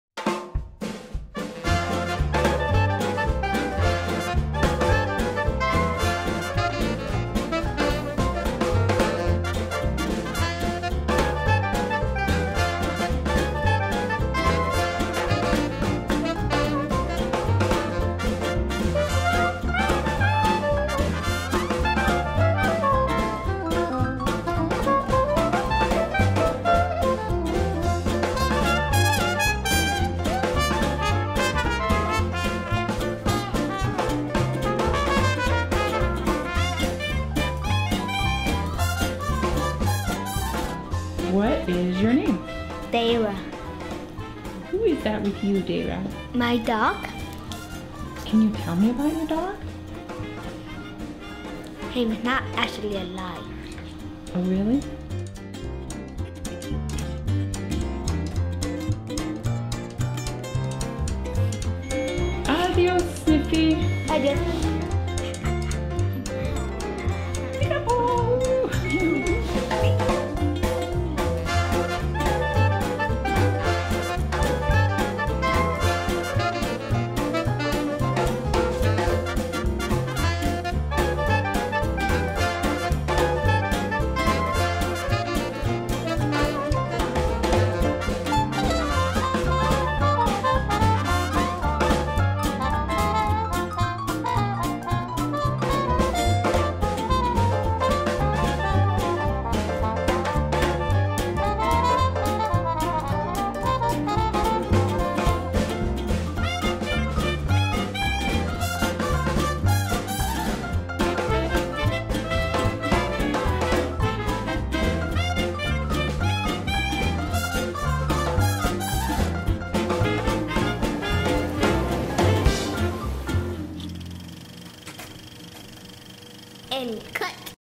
Deyra's Magic Bed
You never know what kind of fun can be found when a camera comes to visit you in the hospital! This little filmmaker shows just what is possible. Great job, Deyra!